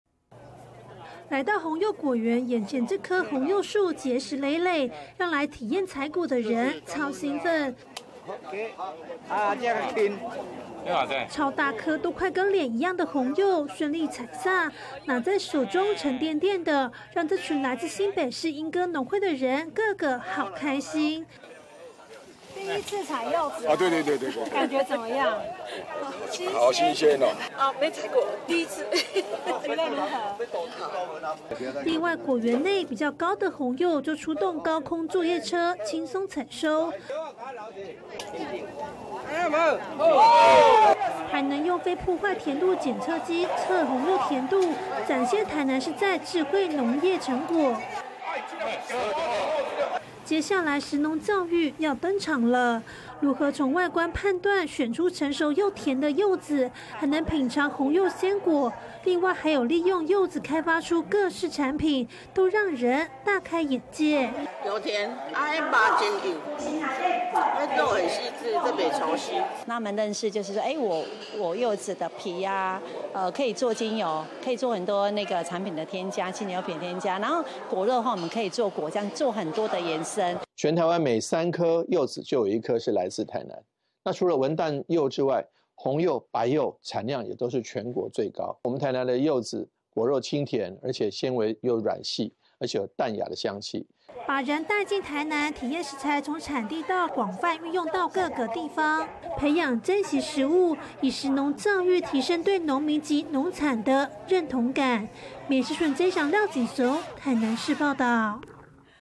遊客走進果園採紅柚！ 台南柚子精油香氛蠟燭夯－民視新聞
【民視即時新聞】南部中心／曾依翔、廖錦雄 台南報導說到柚子，首先聯想到的產地就是台南，有文旦、紅柚以及白柚！推動食農教育，麻豆農會帶著遊客走進果園，從採果、測糖度到用柚子精油作香氛蠟燭，讓大家了解柚子不只能吃果肉，果皮也是有許多用處，希望藉此讓更多人認識台南的在地物產。